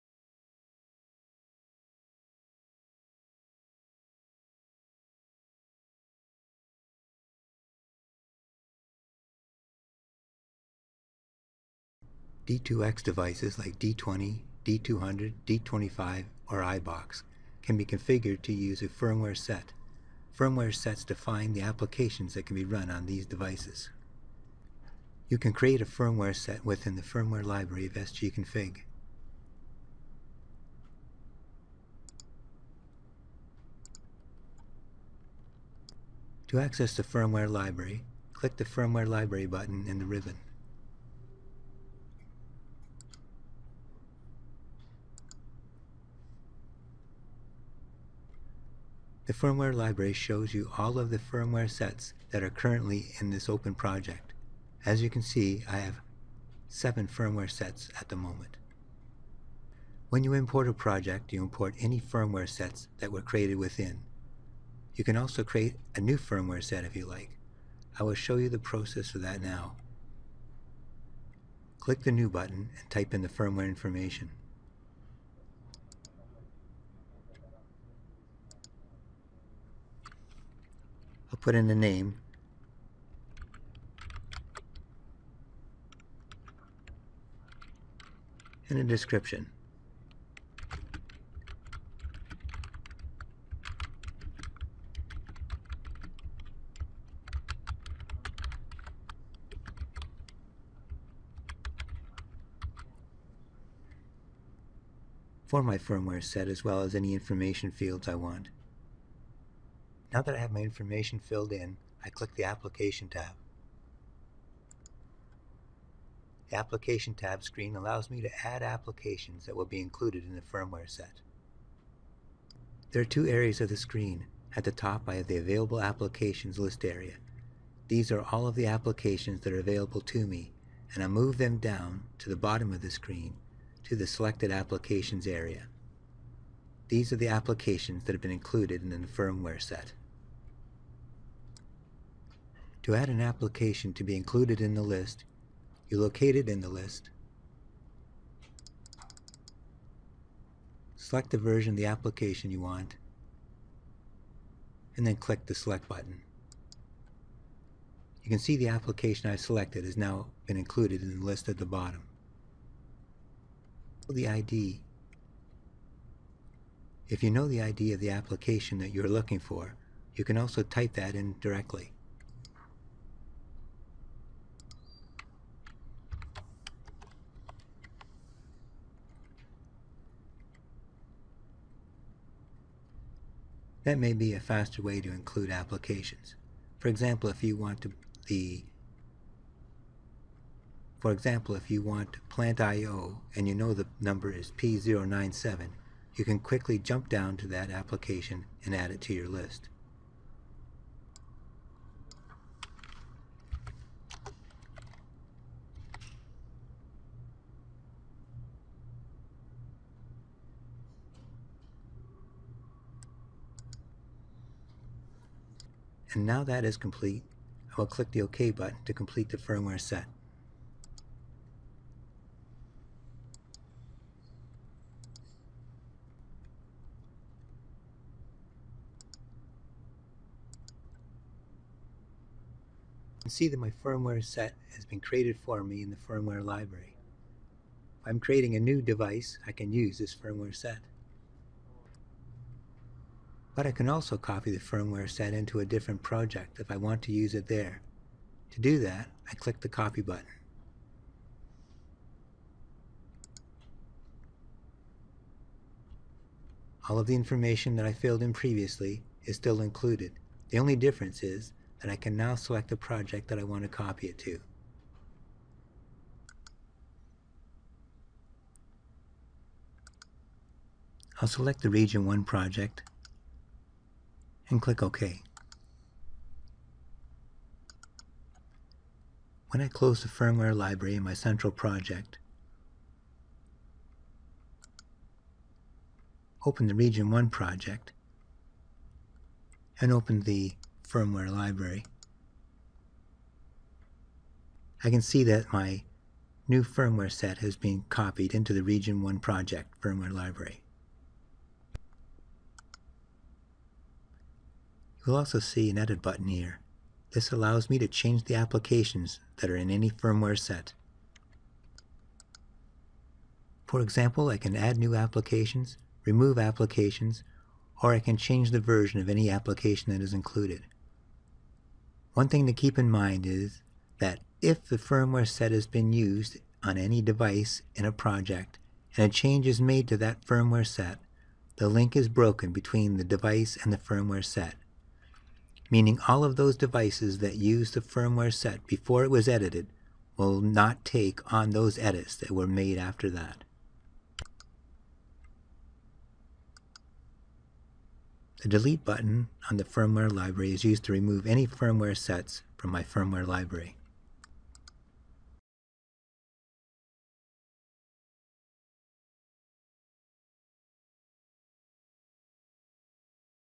CFT-104 l SG Config Firmware Library v1
This module shows how certain devices like D20, D200, D25 and iBox can be configured to use a certain Firmware set.